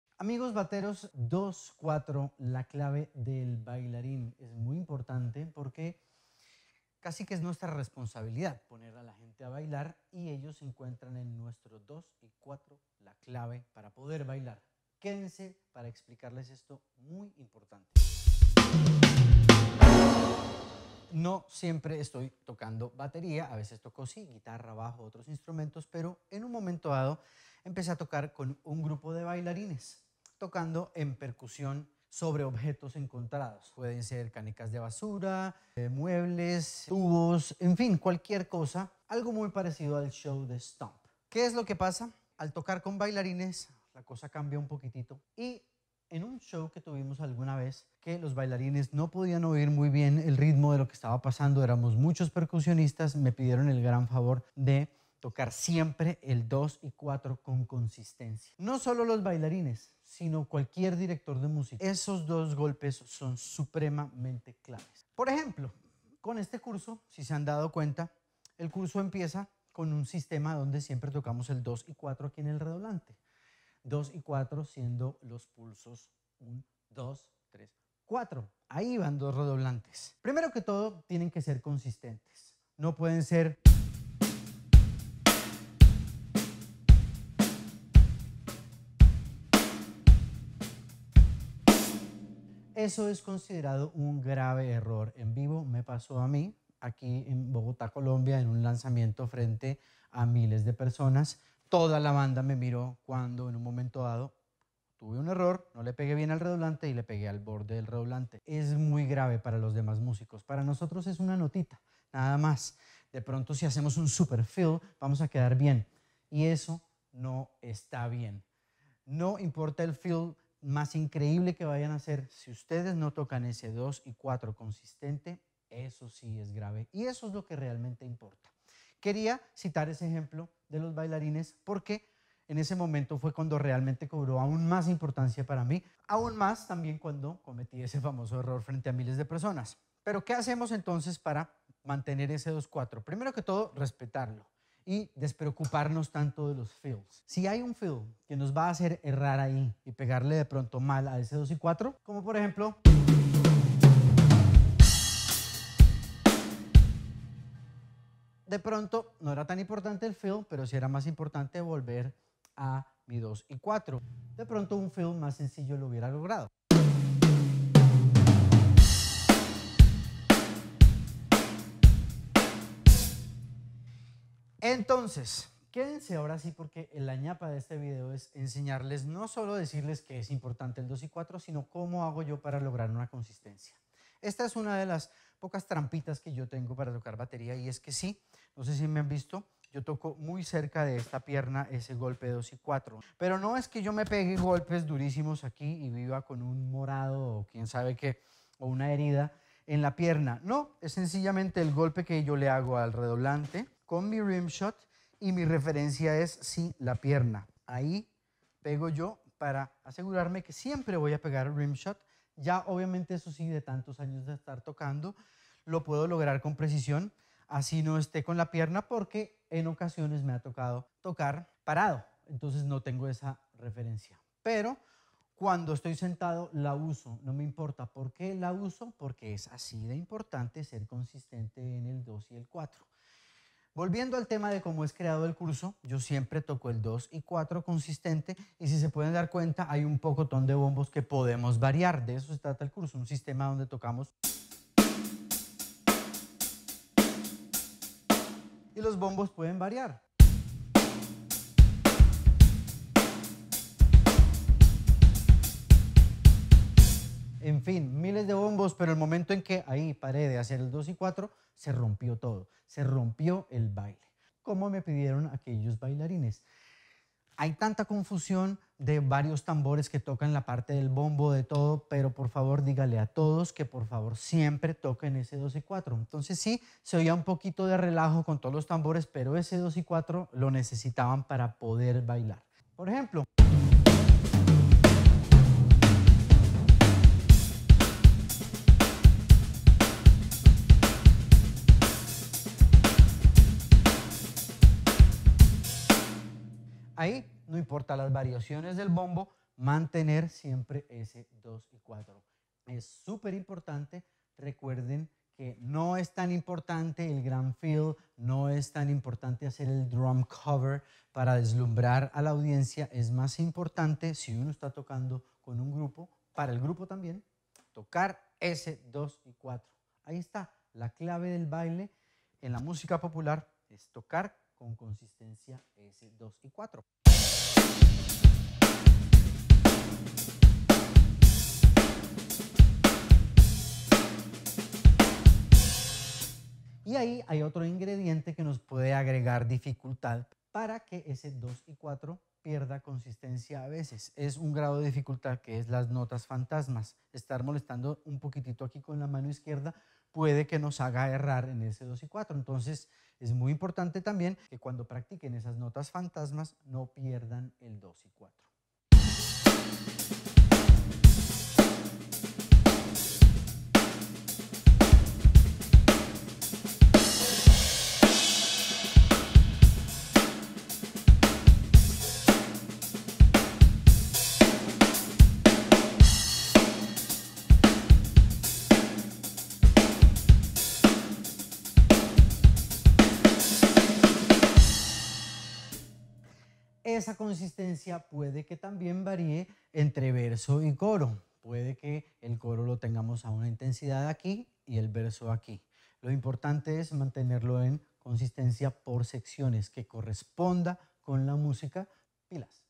Tocar Batería - 2 y 4 consistentes
Tutorial de Batería -  El golpe de redoblante en la música popular y su importancia

Como tocar Batería de Cero.  Sin saber leer o escribir música.